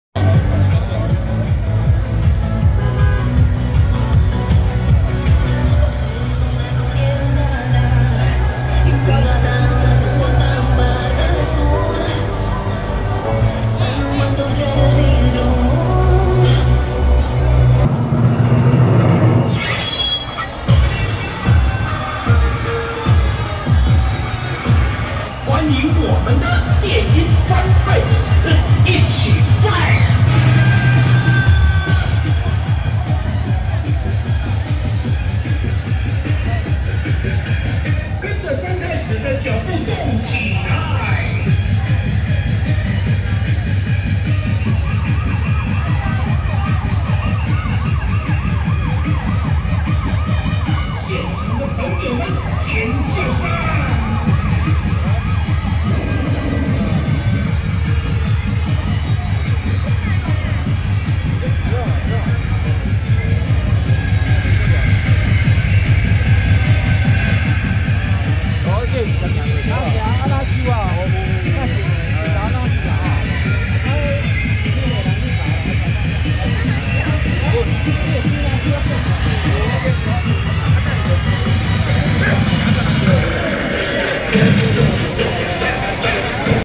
【三重武聯堂~太子咩(千芸、黑黑)＆電音三太子＆大猩猩】~113台北國際帝君會＆閻羅天子聯誼會三年圓科南巡進香回駕遶境
三重武聯堂~太子咩(千芸、黑黑)＆電音三太子＆大猩猩